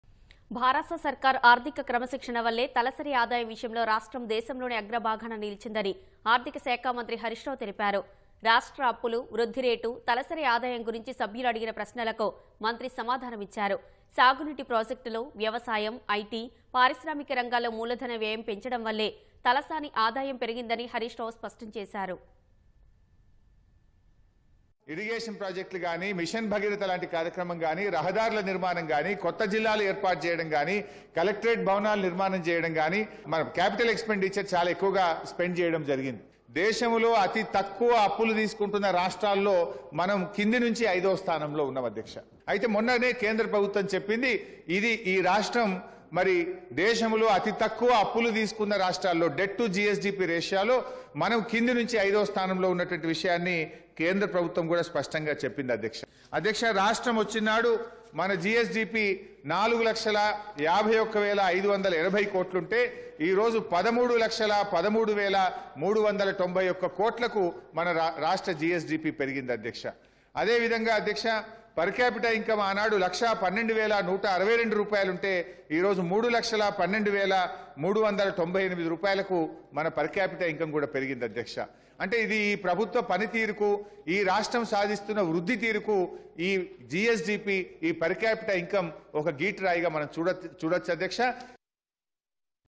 Per Capita Income in Telangana Increased | Thanks to BRS Govt Financial Discipline | Harish Rao
భారాస సర్కార్ ఆర్థిక క్రమశిక్షణ వల్లే తలసరి ఆదాయం విషయంలో రాష్ట్రం దేశంలోనే అగ్రభాగాన నిలిచిందని ఆర్థికశాఖ మంత్రి హరీశ్ రావు తెలిపారు. రాష్ట్ర అప్పులు, వృద్ధి రేటు, తలసరి ఆదాయం గురించి సభ్యులు అడిగిన ప్రశ్నలకు మంత్రి సమాధానం ఇచ్చారు. సాగునీటి ప్రాజెక్టులు, వ్యవసాయం, ఐటీ, పారిశ్రామిక రంగాల్లో మూలధన వ్యయం పెంచడం వల్లే తలసరి ఆదాయం పెరిగిందని హరీశ్ రావు స్పష్టంచేశారు.